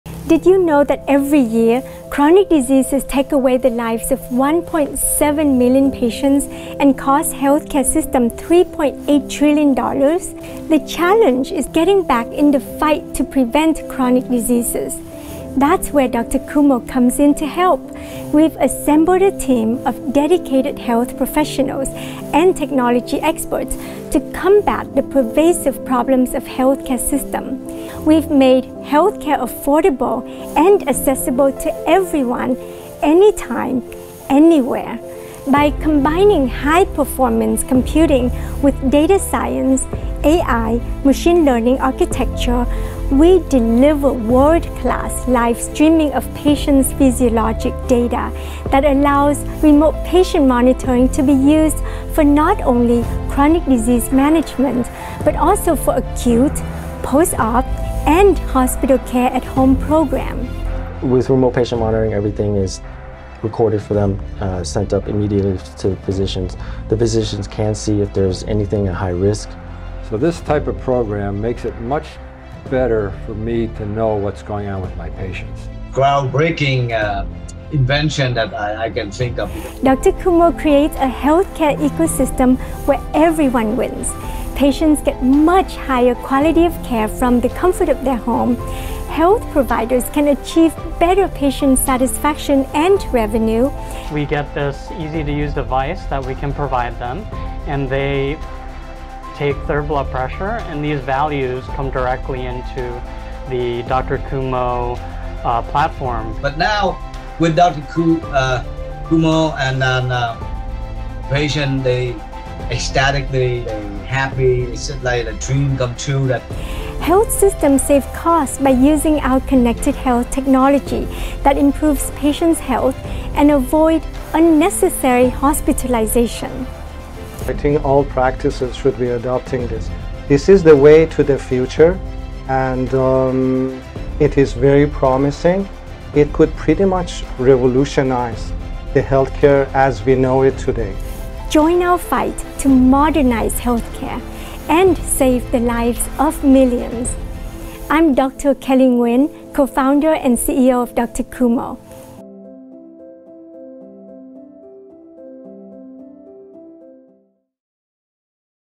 DrKumo: Remote Patient Monitoring for Chronic Disease Prevention and Management
Learn about how remote patient monitoring can help healthcare providers manage chronic diseases and improve patient care. Our technology solutions allow healthcare providers to remotely monitor their patients' health conditions and take timely action when needed.